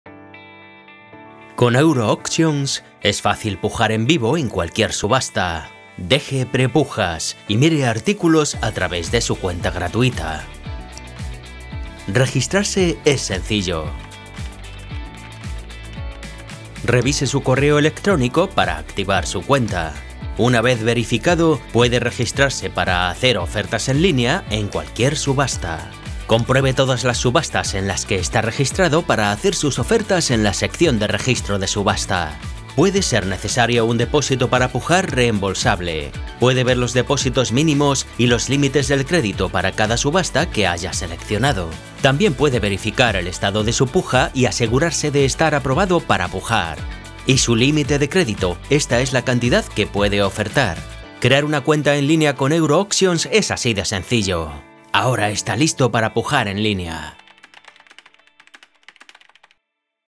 Cree una cuenta en línea y regístrese para una subasta. - Euro Auctions
Aprenda a crear una cuenta en línea y regístrese para las próximas subastas con Euro Auctions. Es rápido, directo y seguro.